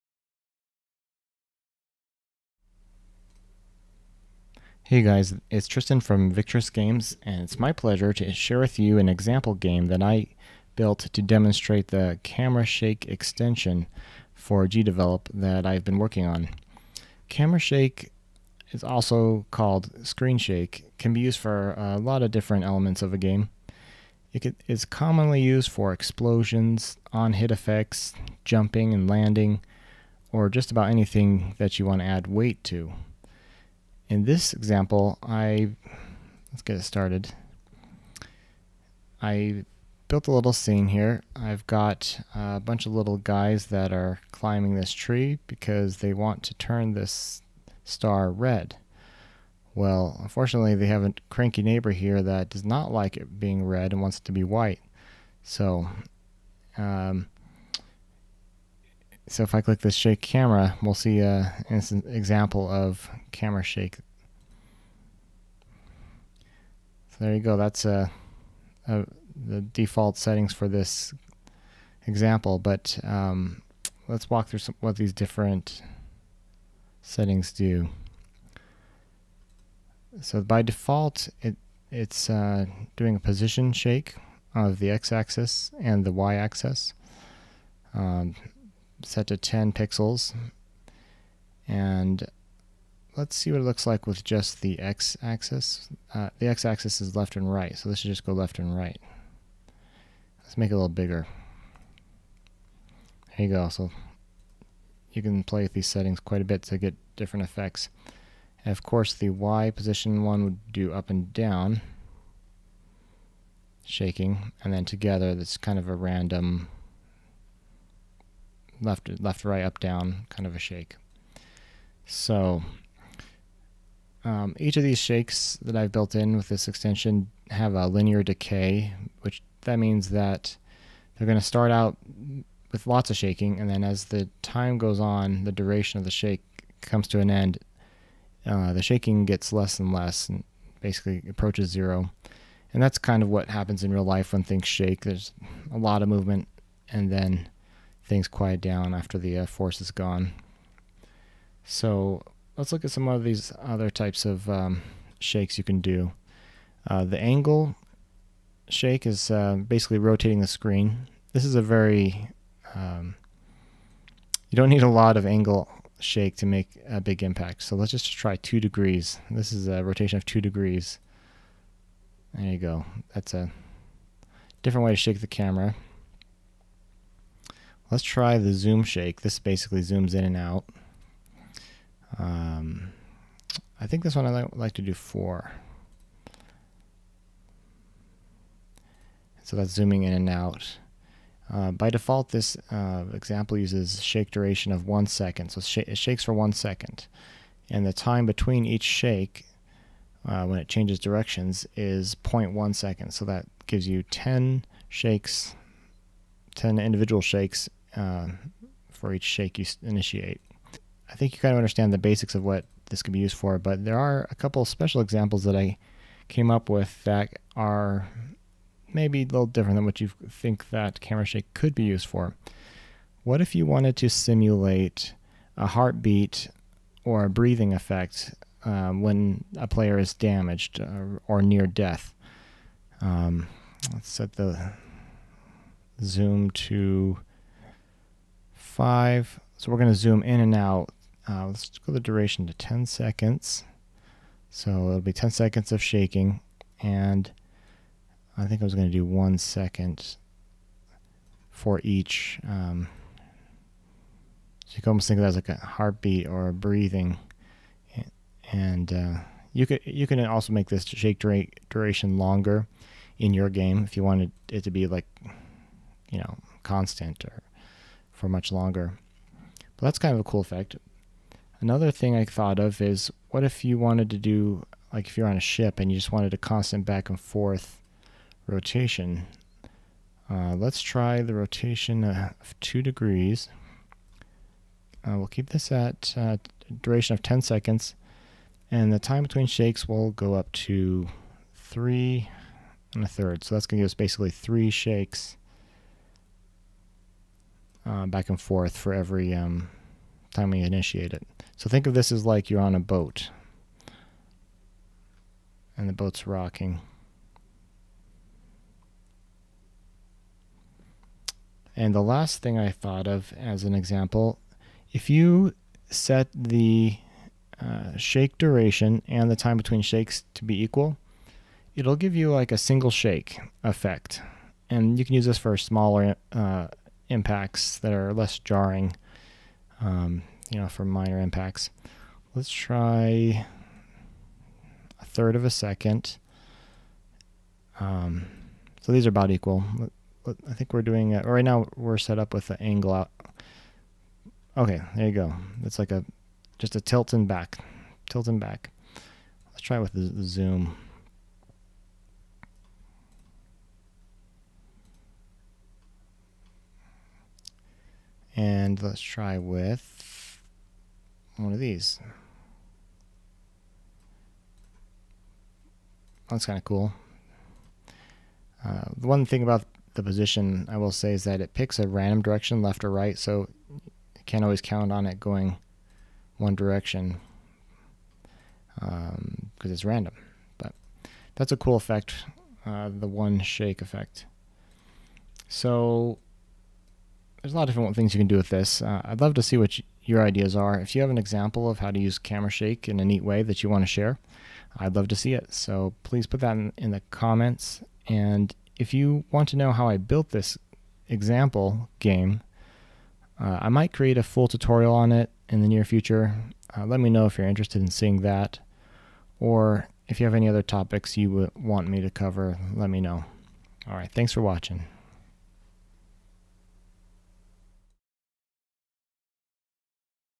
GDevelop: Testing different types of camera shake (screen shake)
Camera shake, also called Screen Shake, can be used for a lot of different elements of a game.  It is commonly used for explosions, on-hit effects, jumping and landing, or just about anything that you want to add weight to.

In this game, I have a bunch of little elves that keep climbing up a tree because they want to turn the star red.  However, the little toy tank over there prefers the star to be white, so he keeps shooting the tree to shake off all the elves.